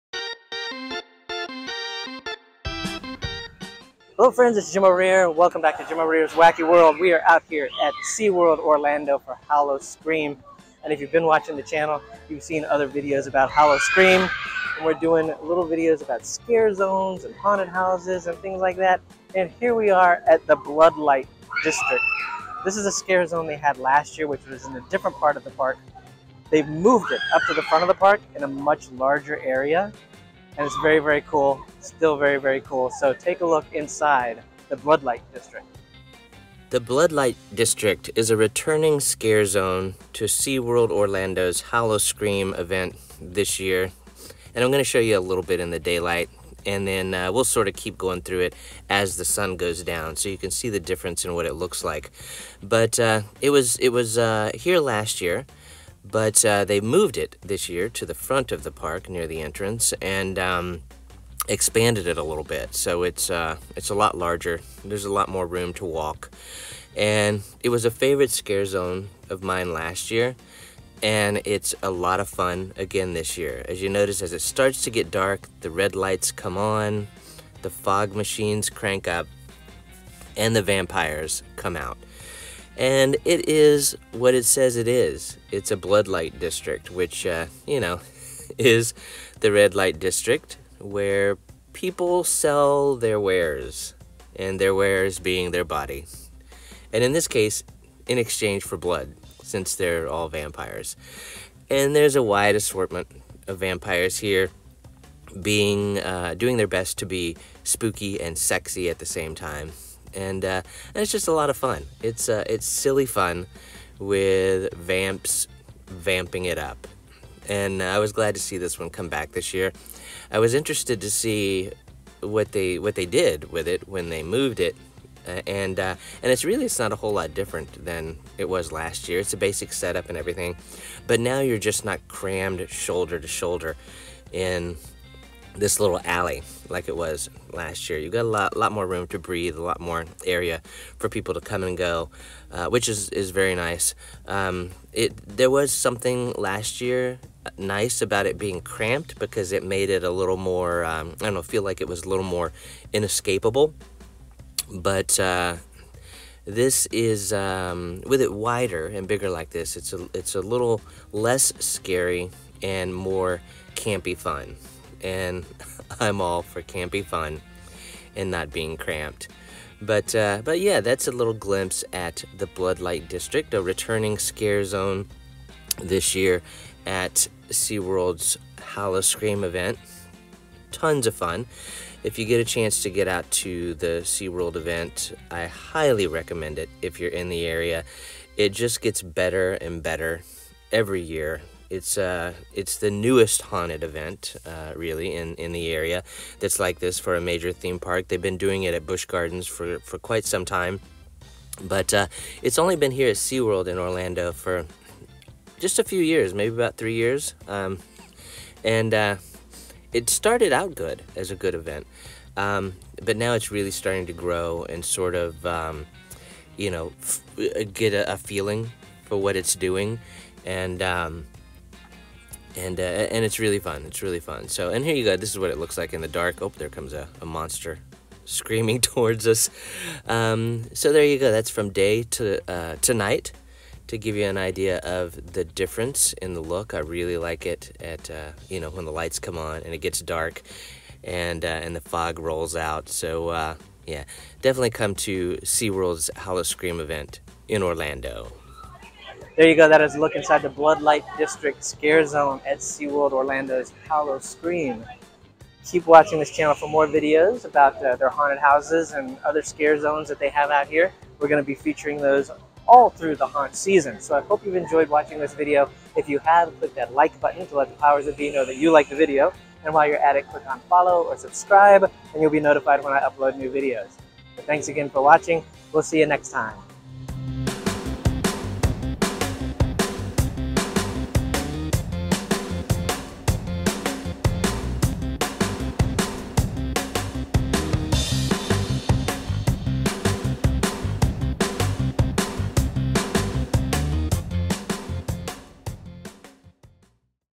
BLOOD LIGHT DISTRICT Sexy Vampire Scare Zone At Howl-O-Scream Orlando!
Step into the BLOOD LIGHT DISTRICT at Howl-O-Scream Orlando! Discover the seductive world of vampires in this thrilling scare zone where creatures of the night rule. With chilling encounters and stunning visuals, this immersive experience brings sexy horror to life. Get ready for a heart-pounding walk through the dark streets filled with alluring, bloodthirsty beings. Don't miss this must-see scare zone!

CHAPTERS:
00:00 Introduction
00:36 Blood Light District Tour
04:08 Recap